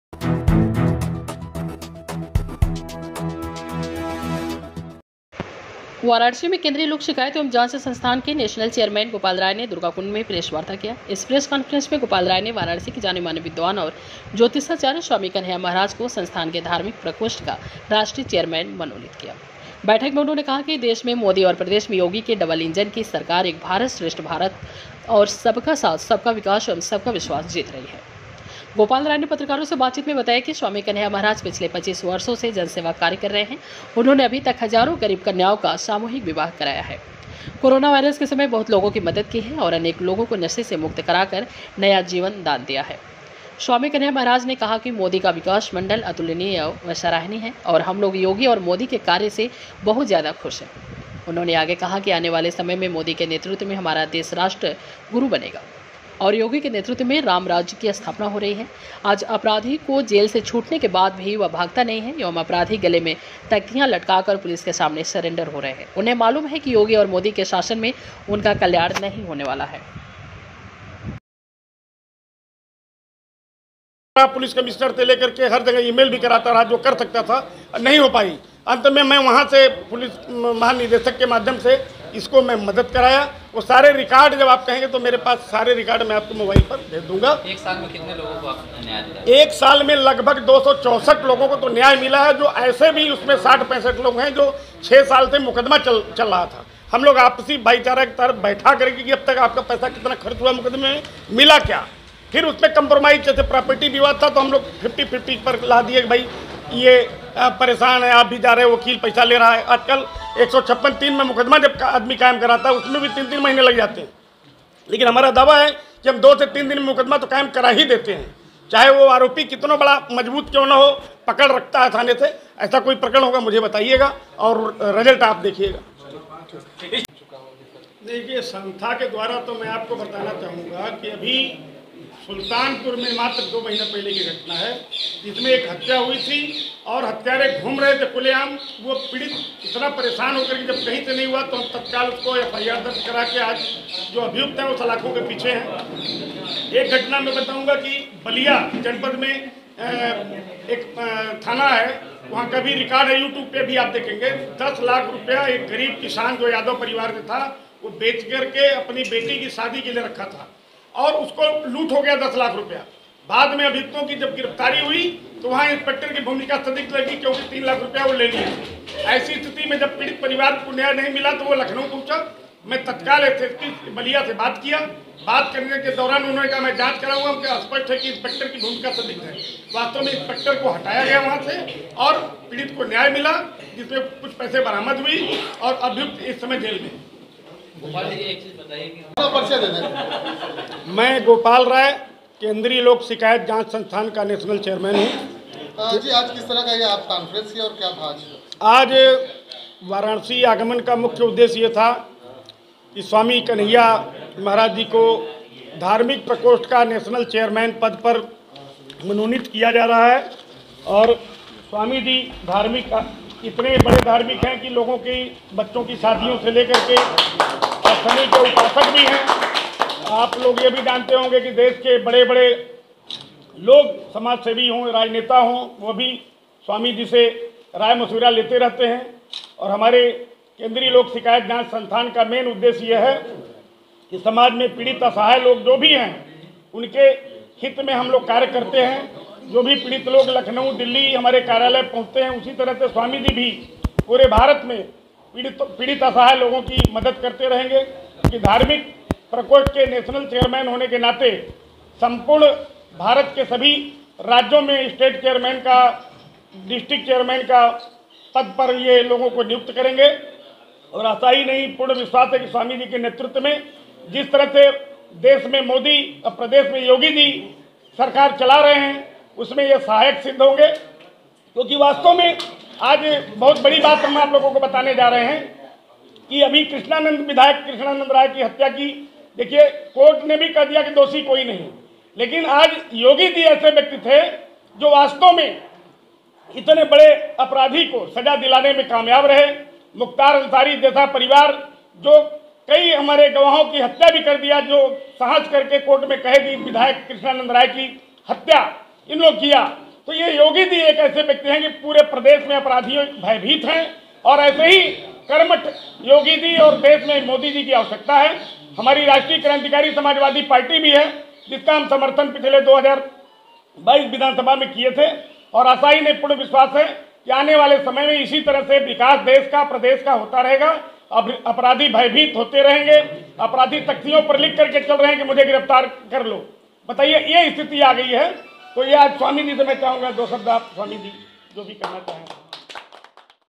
स्वामी कन्हैया महाराज को बड़ी जिम्मेदारी,केंद्रीय लोक शिकायत एवं जांच संस्थान में मिला ये दायित्व
Varanasi : स्वामी कन्हैया महाराज को बड़ी जिम्मेदारी, केंद्रीय लोक शिकायत एवं जांच संस्थान में मिला ये दायित्व